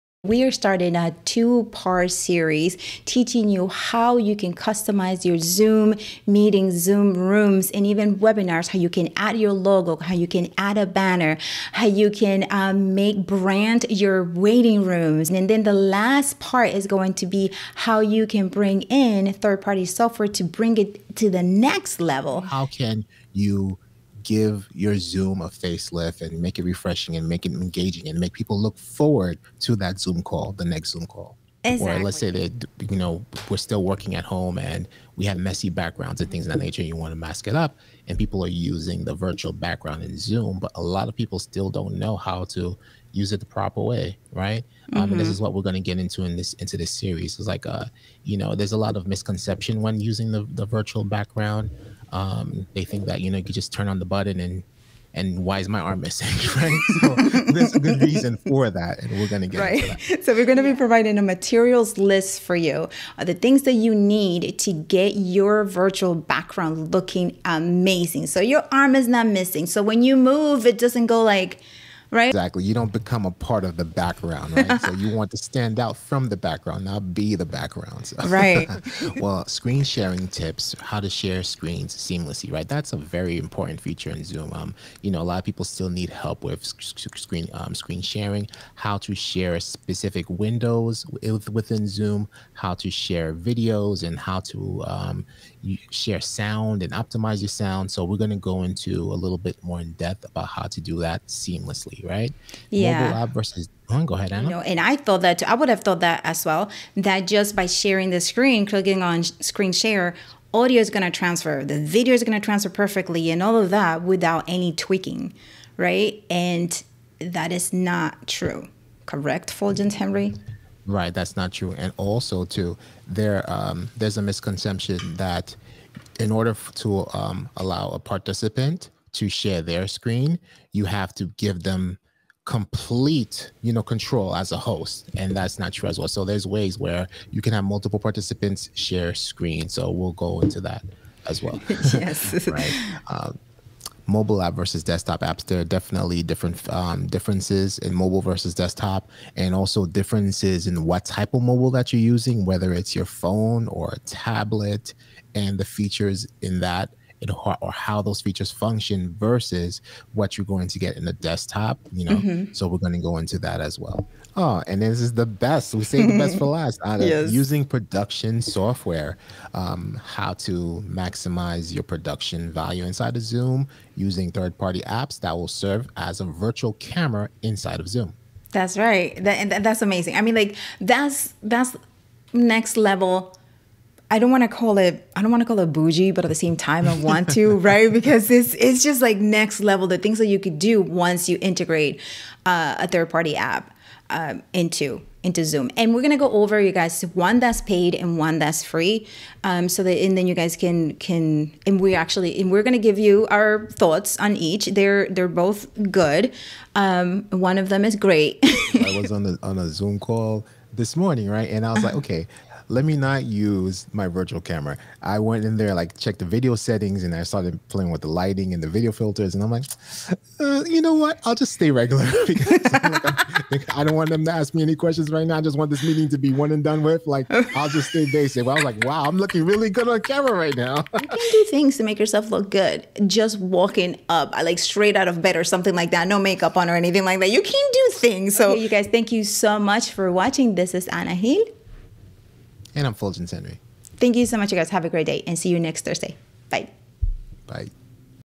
Zoom Series: Virtual Backgrounds and Branding
This summer ana gil of Officially-Unofficial is bringing you a two-part series on Customizing your Zoom Rooms: with guest Fuljens Henry. The first part includes setting up your virtual Backgrounds/green screen; we are going to walk you through the process.

The second part covers: Screen sharing seamlessly and branding your rooms with graphics. This series will give your zoom meetings an amazing face lift!